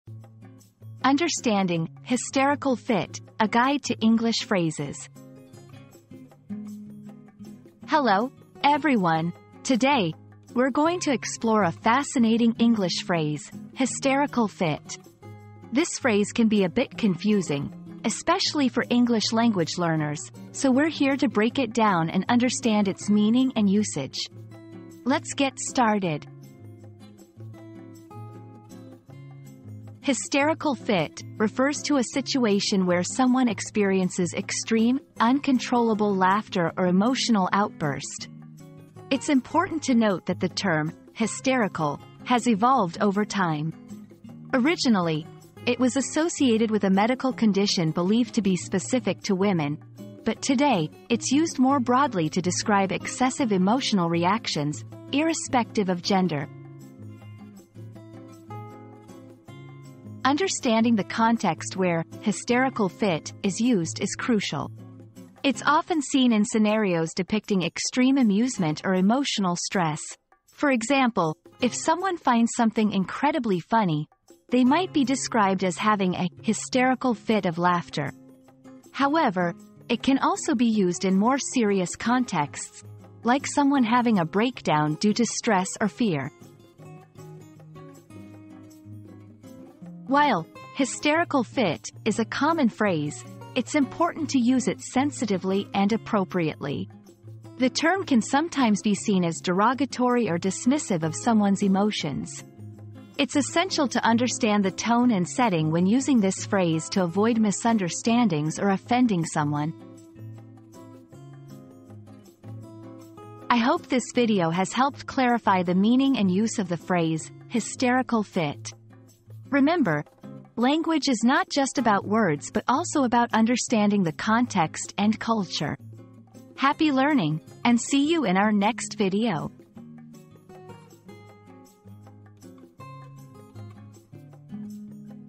Understanding "Hysterical Fit": A Guide to English Phrases
00:00 • Introduction - Understanding "Hysterical Fit": A Guide to English Phrases
00:29 • Defining "Hysterical Fit"
00:59 • Context and Usage
01:29 • Cultural Sensitivity